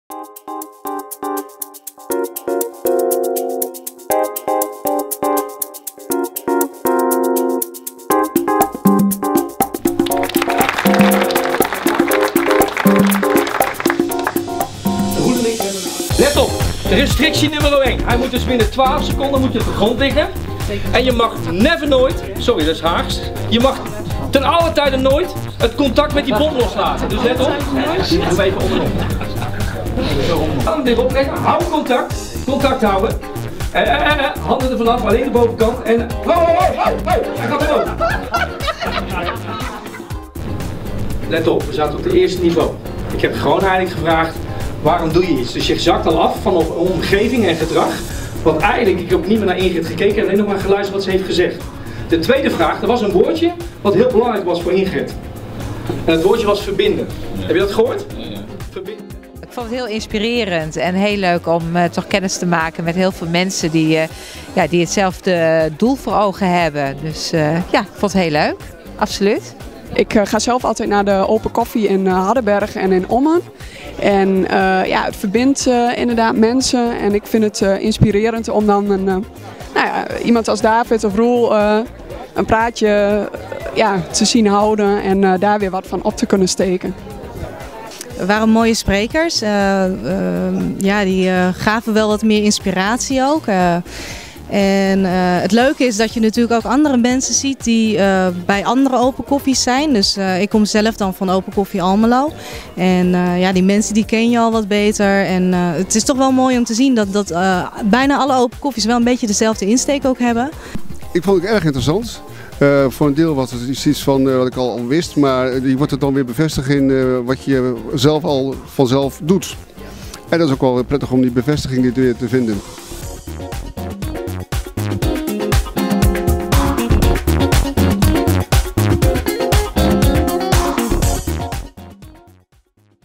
Open Coffee Noord en Oost | 26 maart | Evenementenhal Hardenberg
Voorafgaande aan de vakbeurs Business Inspiratiedagen 2013 was er een Open Coffee bijeenkomst georganiseerd!
(Apeldoorn, Assen, Almelo, Coevorden, Deventer, Enschede, Emmen, Hardenberg, Hengelo, Hoogeveen, Meppel, Ommen, Zwolle)